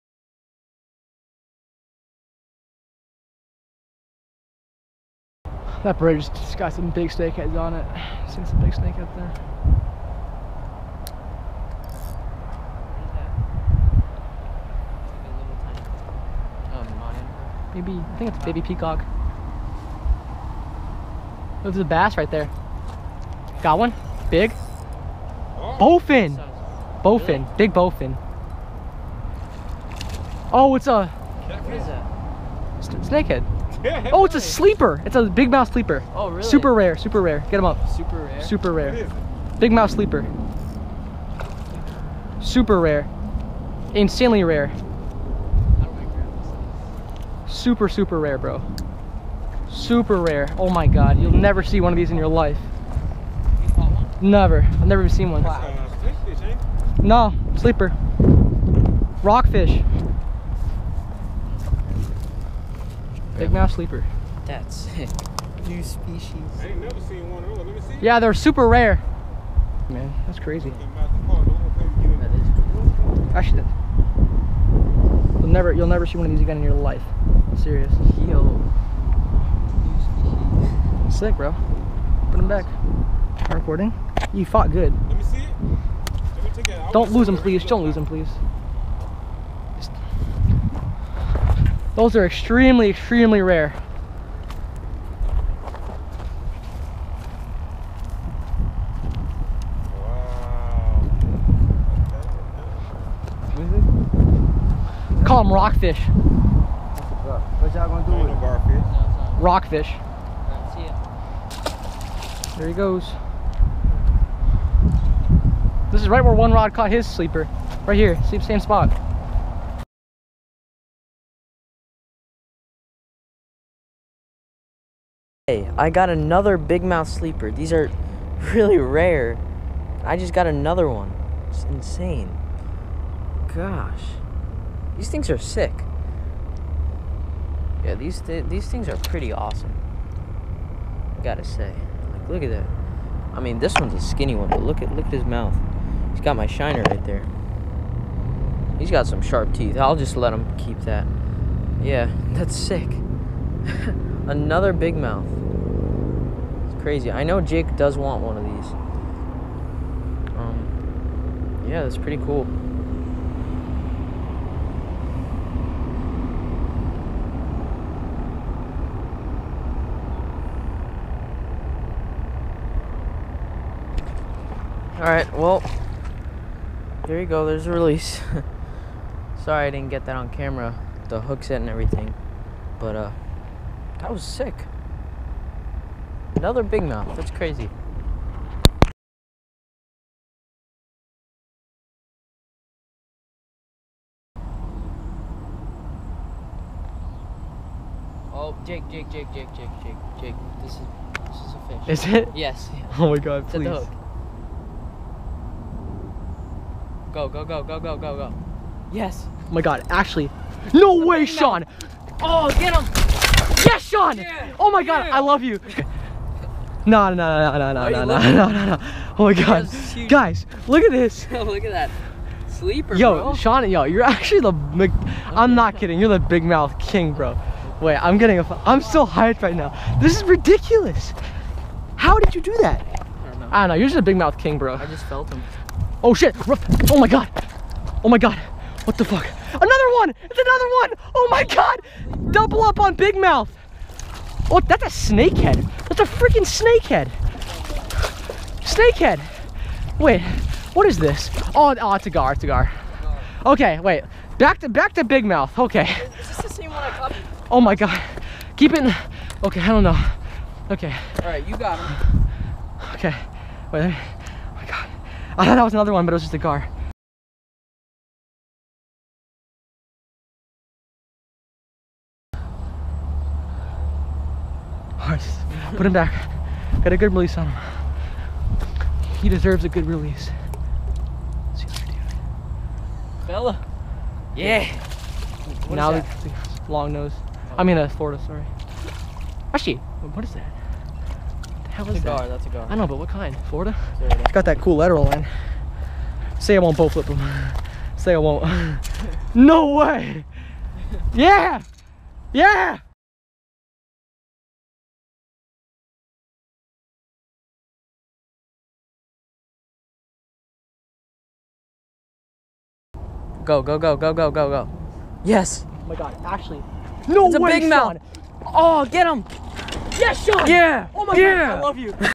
Extremely Rare Bigmouth Sleeper Caught in Florida Canal
Any of you South Florida guys who watch my vids tell me if you have caught any of these? I dont know that many people have caught them so we know of them as a rare fish. Still pretty cool to get my hands on a bigmouth sleeper!